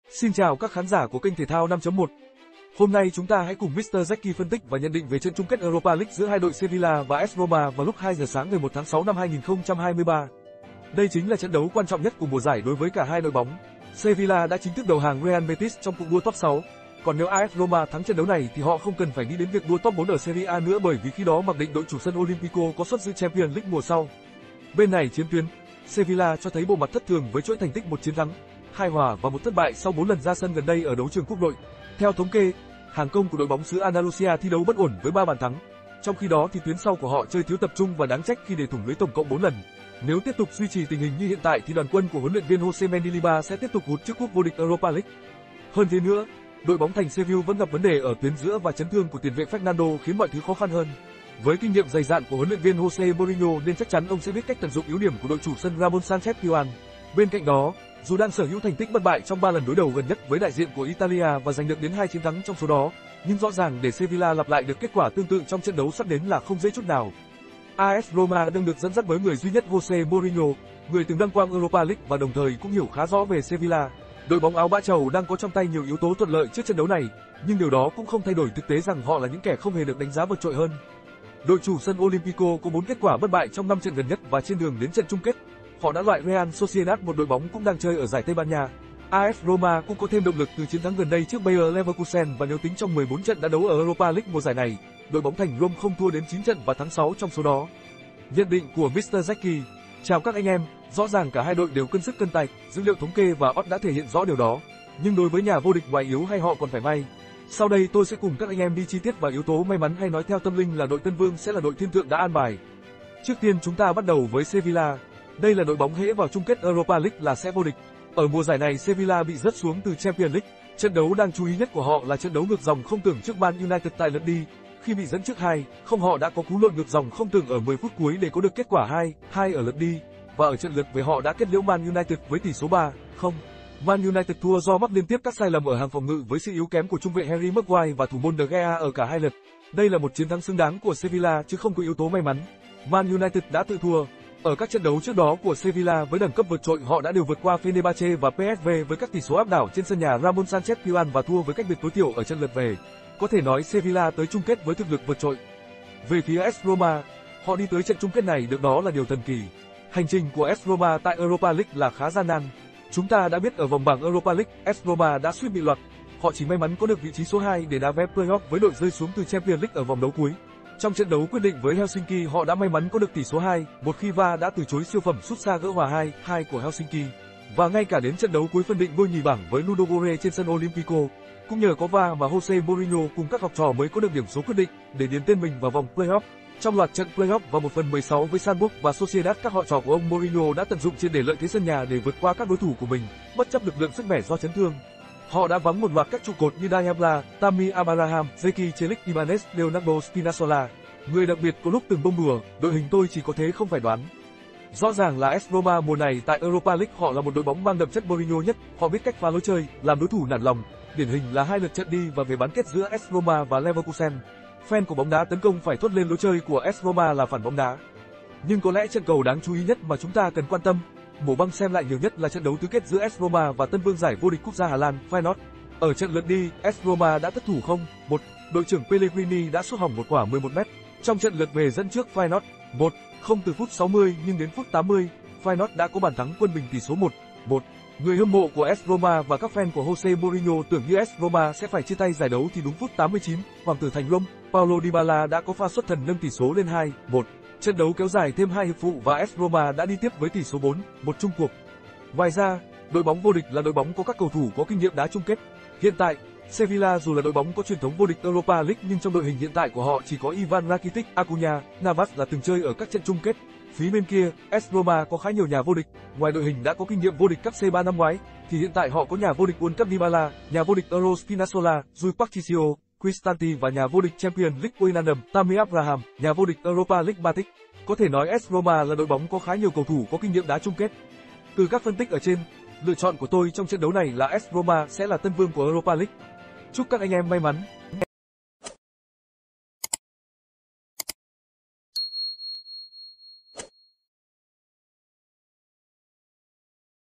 AS ROMA- SEVILLA| CHÂN MỆNH ĐẾ VƯƠNG ĐÃ ĐỨNG VỀ PHÍA ĐOÀN QUÂN CỦA VỊ TƯỚNG TRẬN MẠC MOURINHO?
Hôm nay chúng ta hãy cùng mr Jacky phân tích và nhận định về trận chung kết Europa League giữa 2 đội Sevilla và As Roma và lúc 2 giờ sáng ngày 1/6/2023.